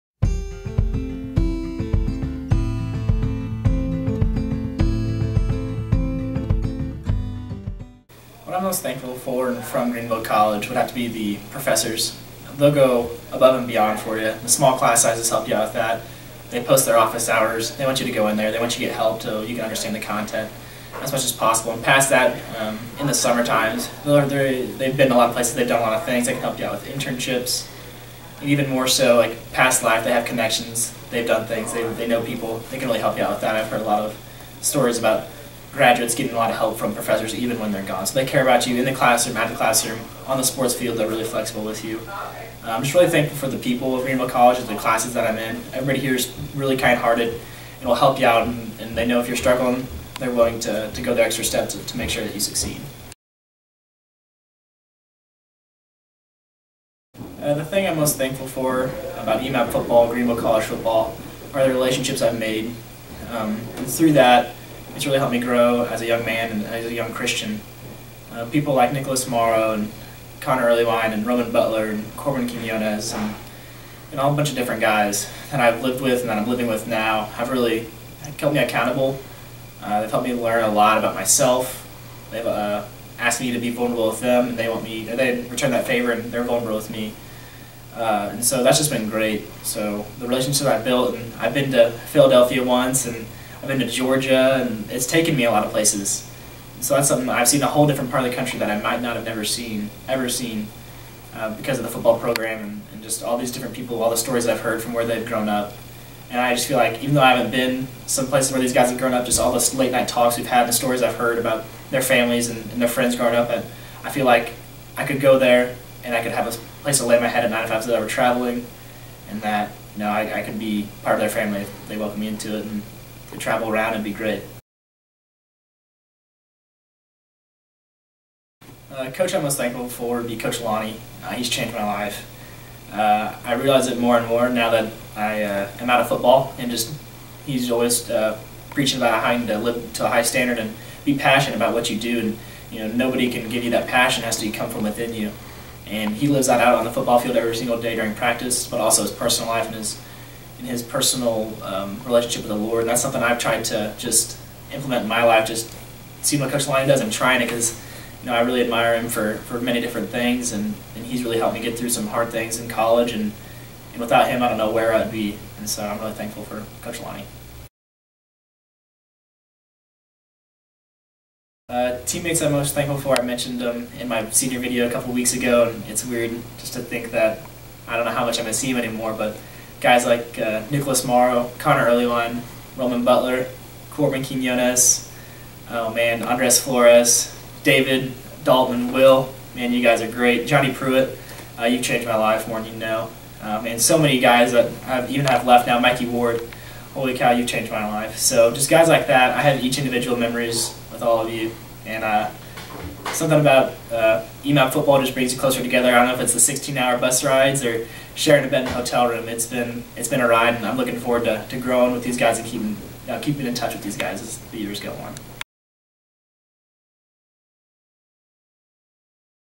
Greenville College and EMAP Football - Cory Snyder Share
2017 Greenville College Graduate Cory Snyder shares what he is thankful for about GC and EMAP Football.